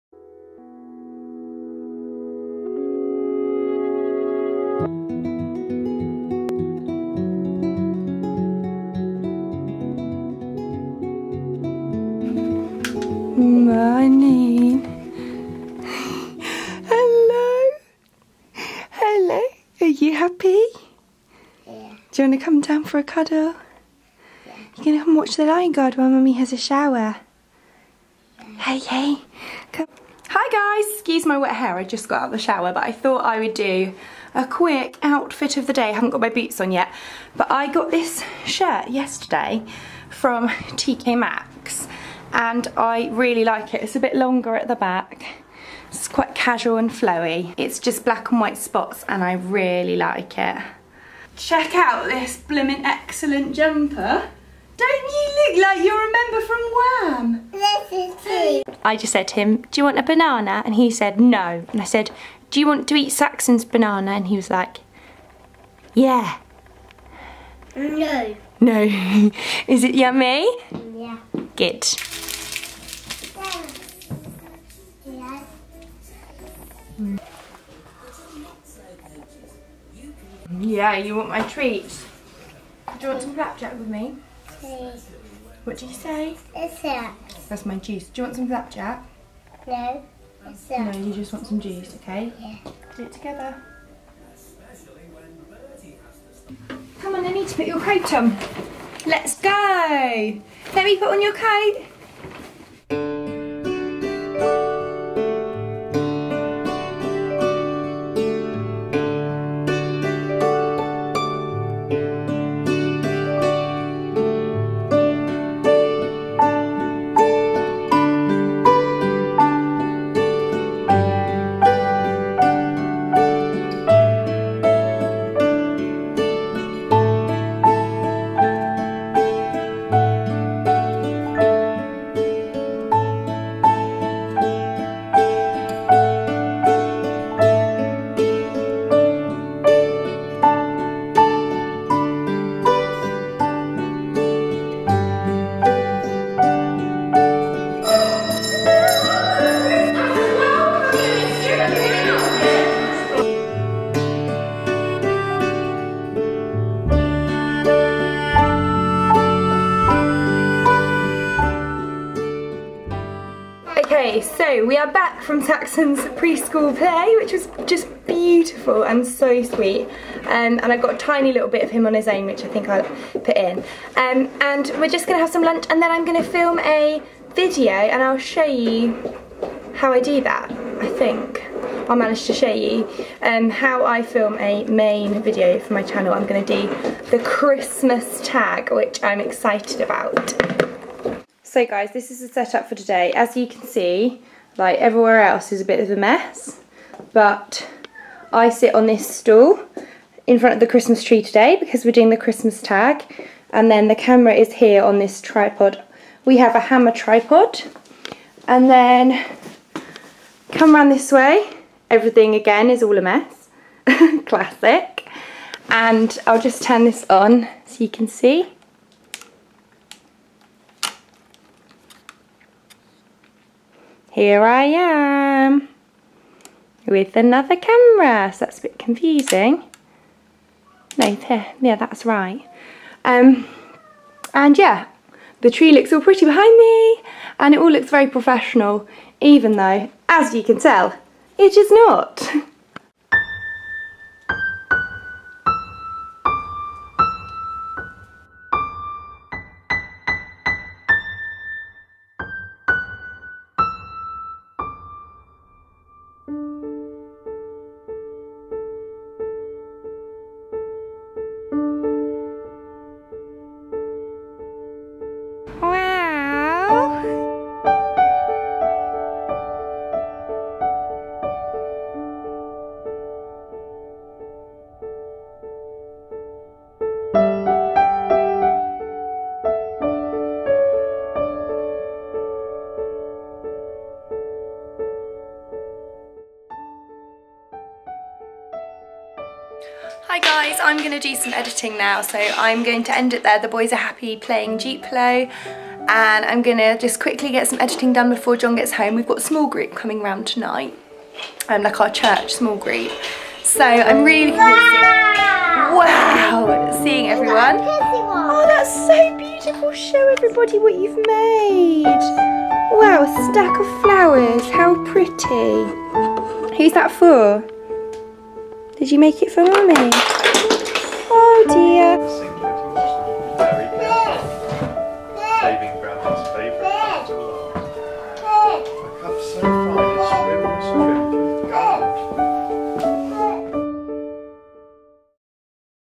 Saxons First School Nativity | RosieSteeleVlogs
Hi Guys,
yesterday we went to saxons first ever nativity and he was just the cutest shepherd I have ever seen! I obviously didnt film other children so there is just a short clip of that. i felt so very proud and had to include it.

we mostly had a day of preschool runs and a bit of a lazy day playing and practising scooting!

Rosie
xxx